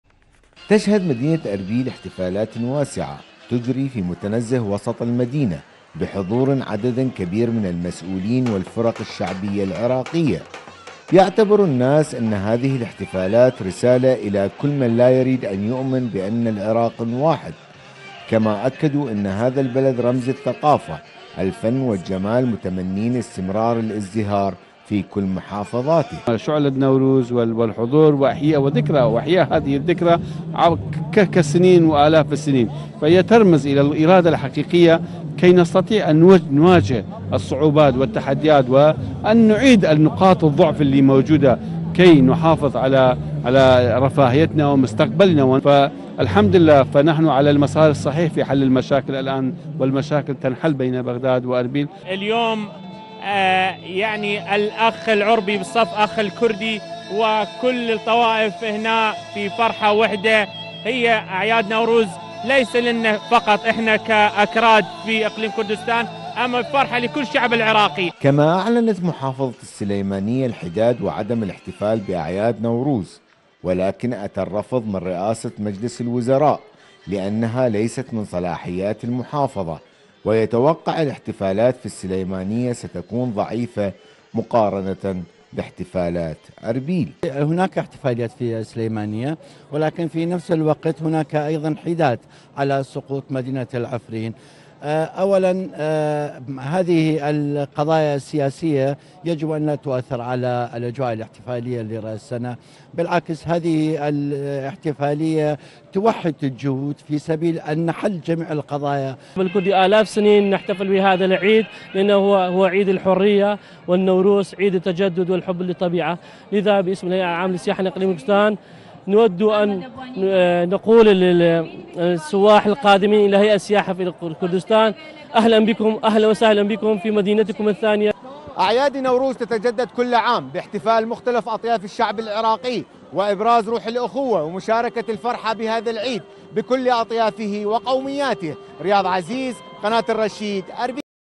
محافظة أربيل الاتحادية تحتفل باعياد نوروز
كما شهدت محافظة أربيل إحتفالات واسعة في المتنزهات بحضور عدد كبير من المسؤولين والفرق الشعبية العراقية.